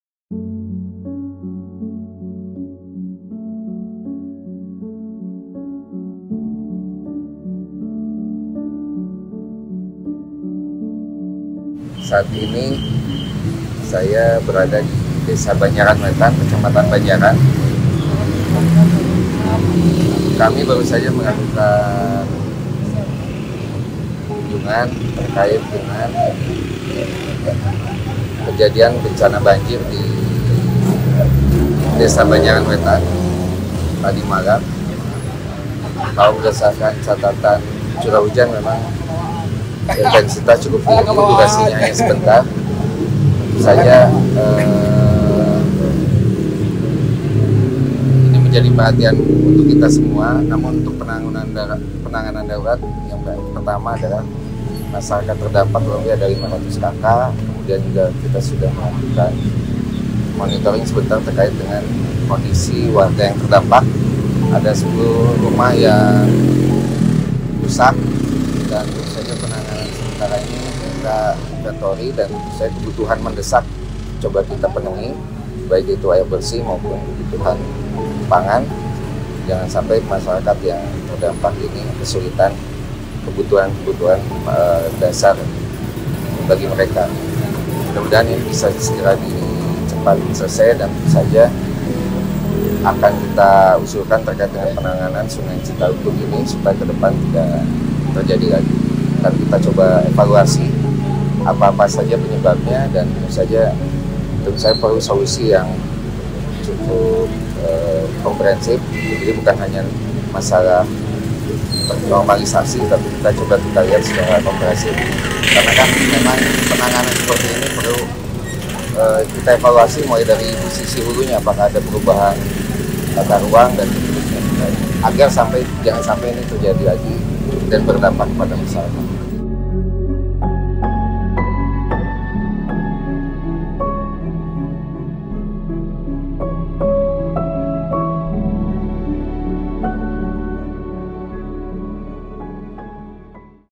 Peninjauan Banjir di Banjaran dan Arjasari
Wargi Kabupaten Bandung…

Pj Gubernur Jabar beserta Pjs Bupati Bandung melakukan peninjauan langsung ke lokasi-lokasi terdampak banjir di Kecamatan Arjasari dan Banjaran. Dalam peninjauannya ia fokus melakukan evakuasi dan asesmen untuk penanganan darurat, diantaranya menyalurkan bantuan dasar logistik kepada masyarakat terdampak.

“Saat ini kami sudah bergerak melakukan monitoring dampak kerusakan dari bencana banjir yang menimpa wilayah Arjasari dan Banjaran. Dan penanganan sementara ini kita inventory kebutuhan mendesak seperti air bersih maupun stok pangan,” ujar Pjs Bupati.

Berdasarkan data sementara, Pjs Bupati menginformasikan terdapat sebanyak 500 kartu keluarga dan 10 rumah terdampak bencana yang mengalami kerusakan.

Selain Kecamatan Arjasari dan Banjaran, dampak hujan deras yang disertai angin kencang ini menimpa beberapa wilayah di Kabupaten Bandung diantaranya, Kecamatan Rancabali, Pameungpeuk, Pasirjambu, Katapang, Cimaung, Cangkuang, Ciwidey, dan Bojongsoang.

Videografer: @qihfaqiih